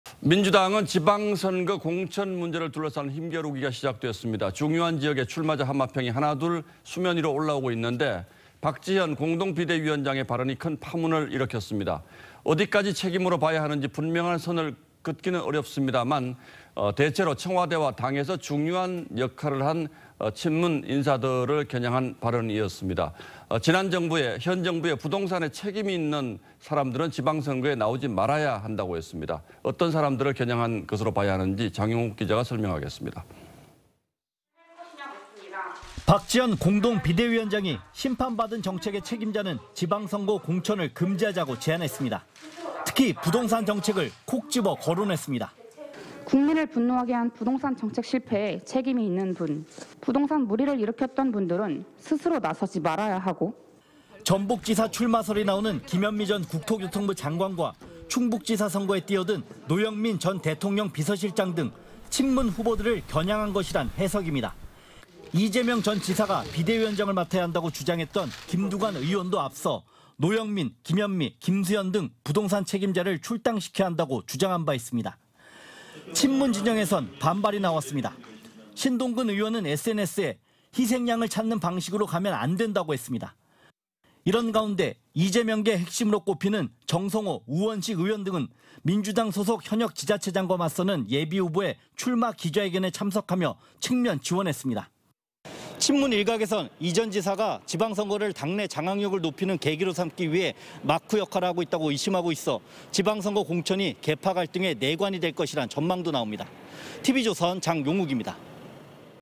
박지현 "부동산 실패 책임자 공천 금지"…친문-친명 갈등 조짐
민주당은 지방선거 공천 문제를 둘러싼 힘겨루기가 시작됐습니다. 중요한 지역의 출마자 하마평이 하나 둘 올라오고 있는데 박지현 공동비대위원장의 발언이 큰 파문을 일으켰습니다. 어디까지 책임으로 봐야 하는지 분명한 선을 긋기는 어렵지만 대체로 청와대와 당에서 중요한 역할을 한 친문 인사들을 겨냥한 발언이라는 해석이 나왔습니다.